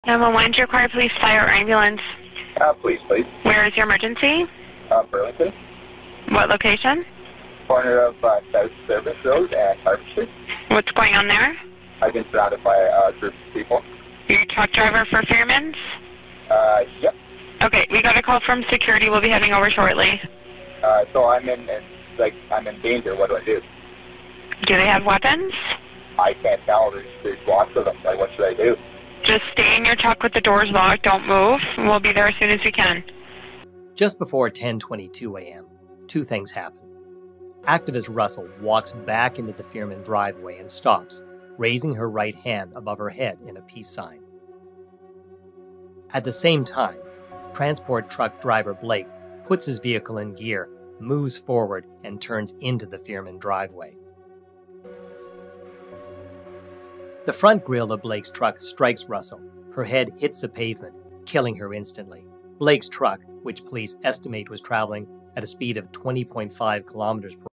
Investigation: 911 Told Driver Not To Move Before Killing Activist
A new Toronto Star investigation reveals that the driver who struck & killed animal advocate Regan Russell outside Fearmans pig slaughterhouse in Burlington, Ontario, was told by 911 not to move right before he ran her over. This critical information was never shared in the court case against the driver.

Shocking new surveillance video shows Russell standing in full view of the truck while she is plowed down.

The driver pleaded guilty to careless driving causing death under the provincial Highway Traffic Act, escaping criminal prosecution. He was ordered to pay $2,000—the minimum possible fine.

Provincial charges are considered far less serious than criminal charges, with significantly lower penalties for those convicted, & no criminal record. A comparable criminal offence, such as dangerous driving causing death, would be punishable by large fines & up to 14 years in prison, plus a criminal record.

Although the police did not lay criminal charges against the truck driver, they rarely extend this leniency to animal advocates. Law enforcement authorities often give slap-on-the-wrist treatment to industries responsible for animal suffering, while criminally prosecuting & seeking to jail people who take action to stop animal cruelty.